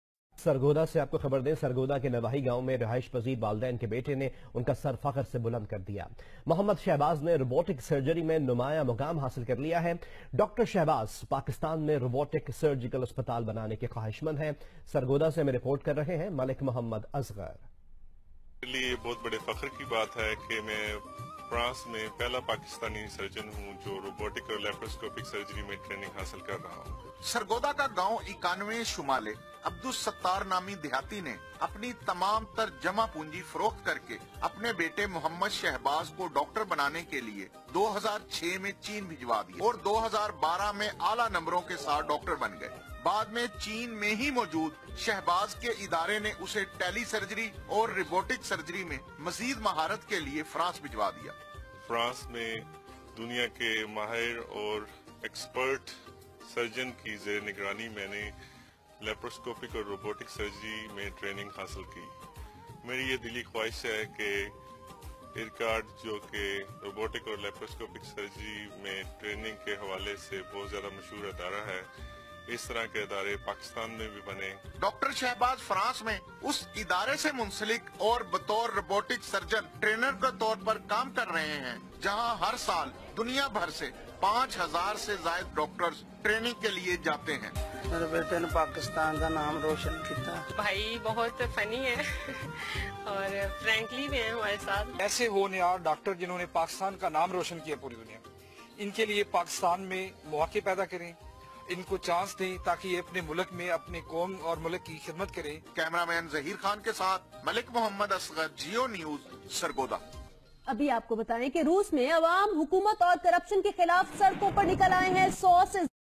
Geo tv Dr.Muhammad Shahbaz on Geo TV. First Pakistani Fellow at IRCAD France
Dr.Muhammad Shahbaz on Geo TV. First Pakistani Fellow of Minimally invasive & Robotic surgery at IRCAD France